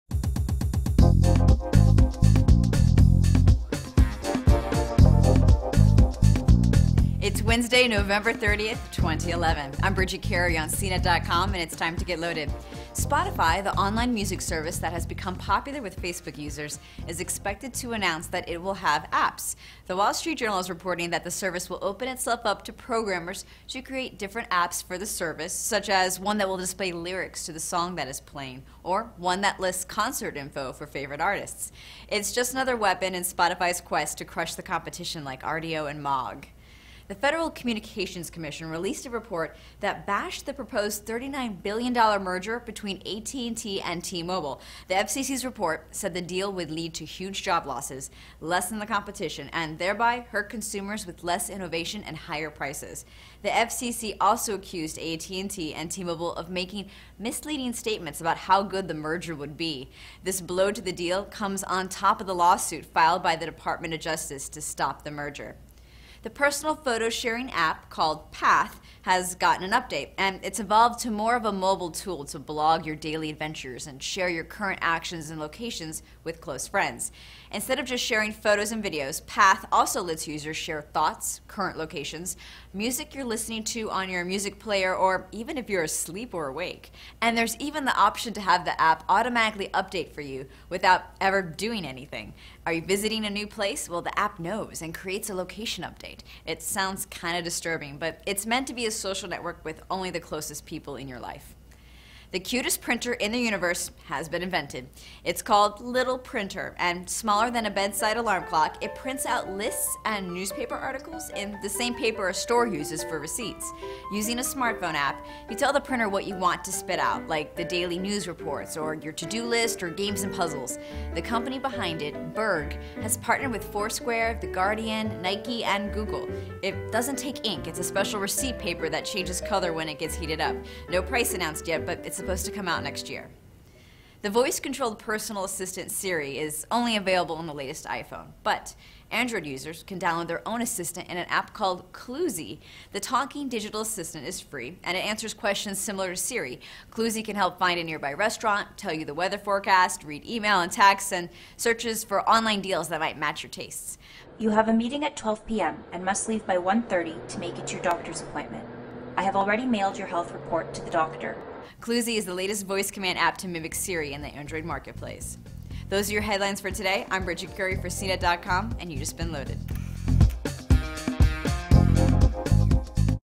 Loaded: Spotify adds app ecosystem
The FCC slams the AT&T and T-Mobile merger, the cutest printer in the world gives bite-sized news reports, and Spotify hopes to one-up the competition with music-related apps.